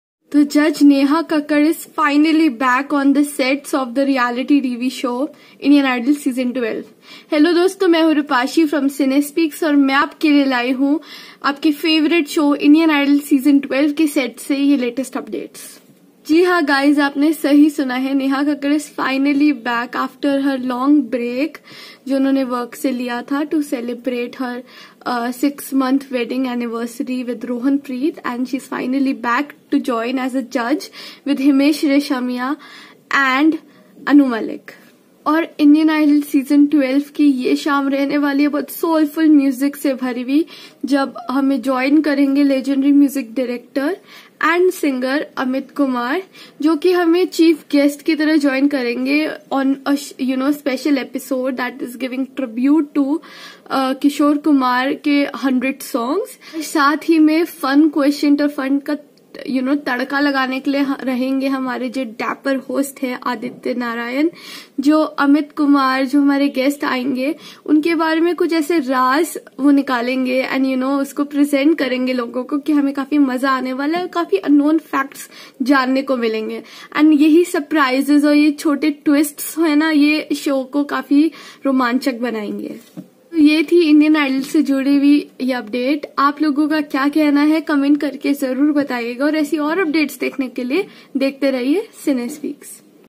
After a long break, Neha Kakkar is back as a judge on Indian Idol.
kya Neha Kakkar ki ho rh hai wapasi indian idol ke set pe ?